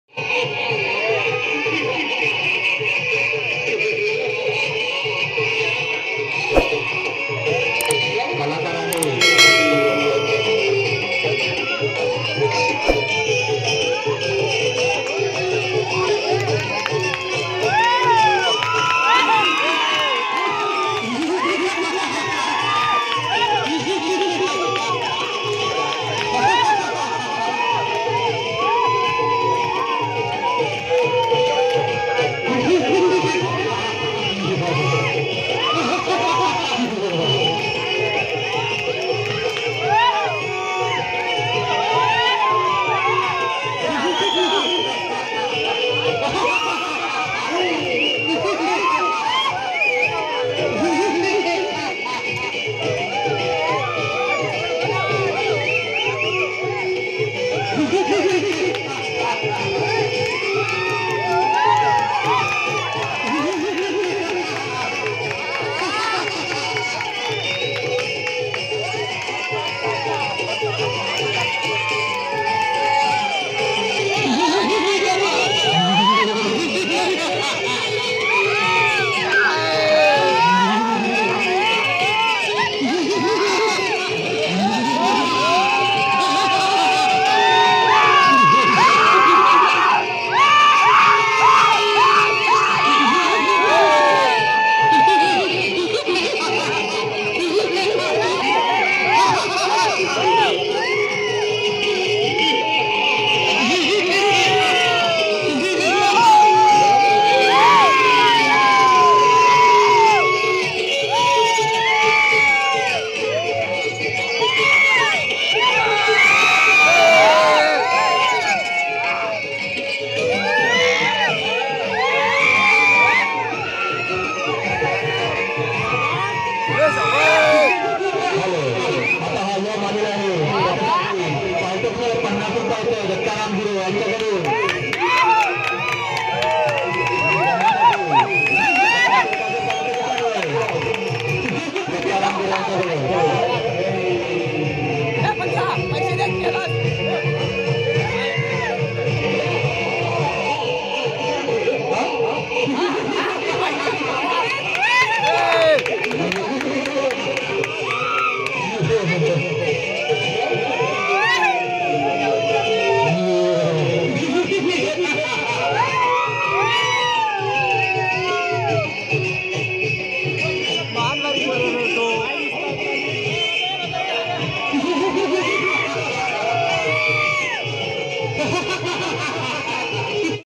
नमन|devendra zhiman naman #2022|बाल कलाकाराने साकारलेली यमाची भूमिका|यम राज एन्ट्री
धन्यवाद🙏